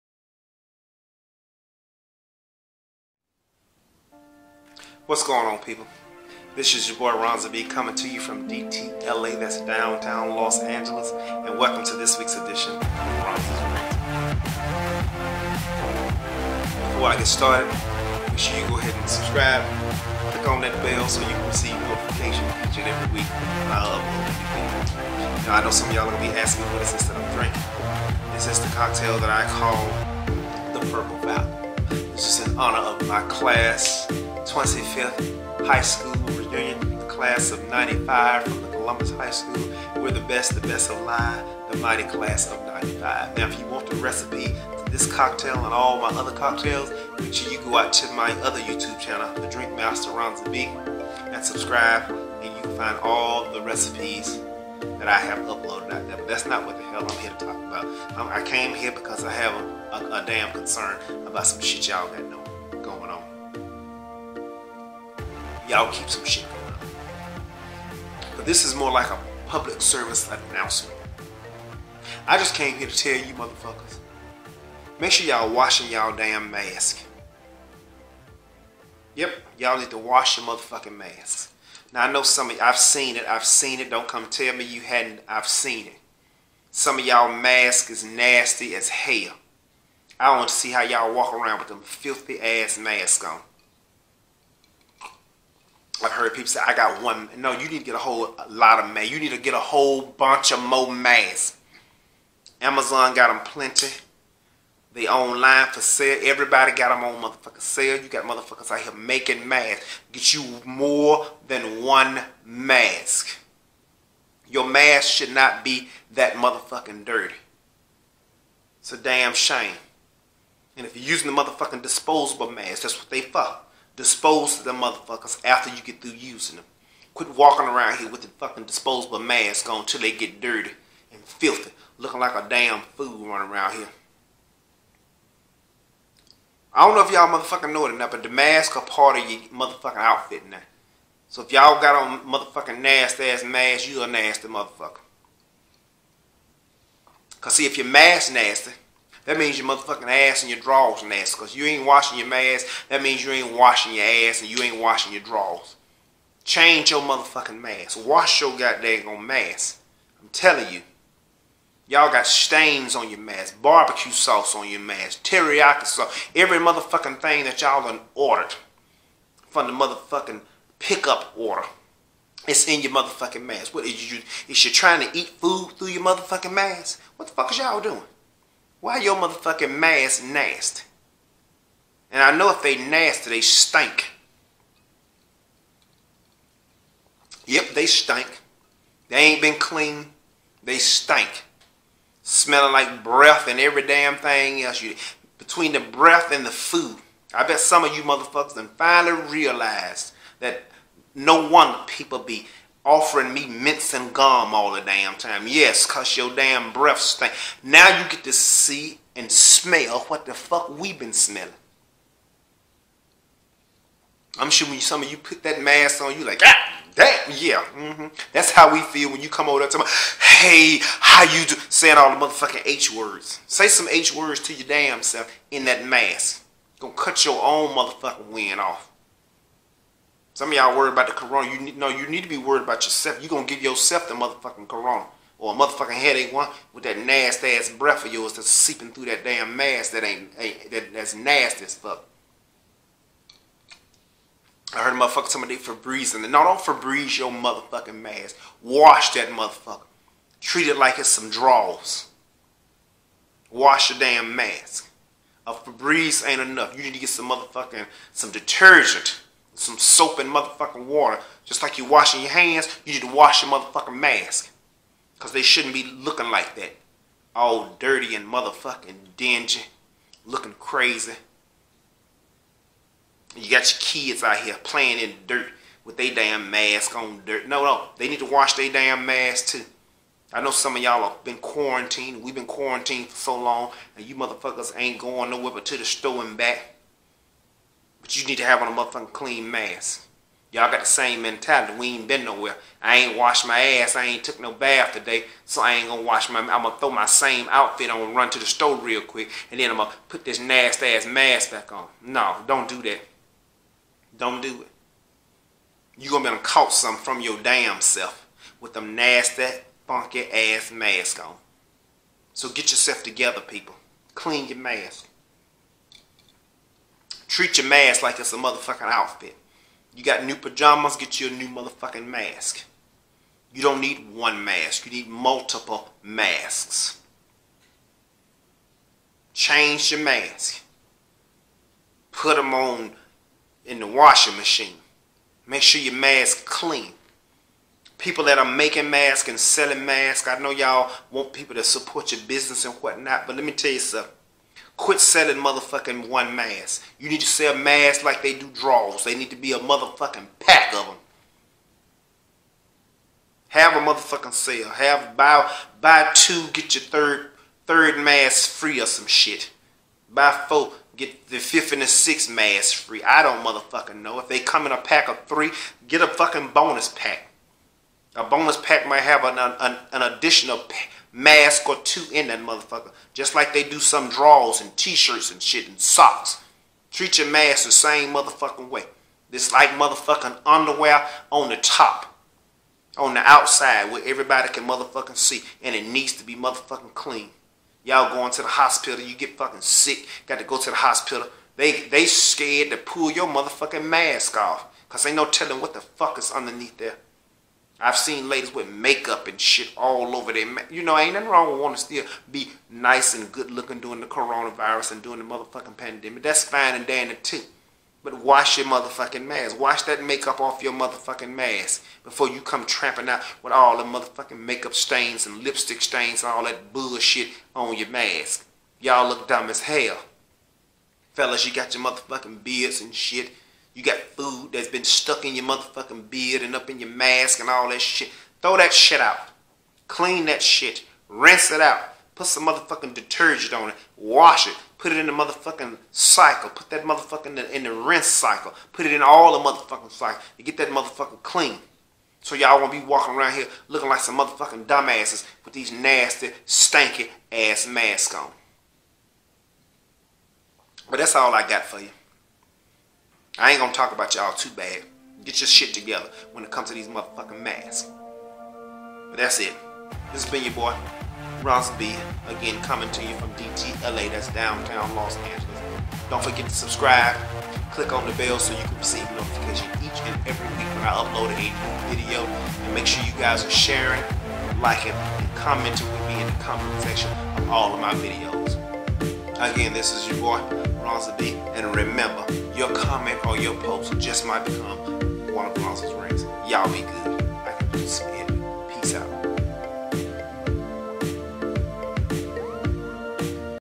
Ronza's Rants - Wash Your Masks! - COVID19
This week on Ronza's Rants, Ronza provides a Public Service Announcement (PS). During this Coronavirus COVID19 Pandemic, he is urging everyone to make sure that they have on a clean mask. Wash Your COVID19 Mask.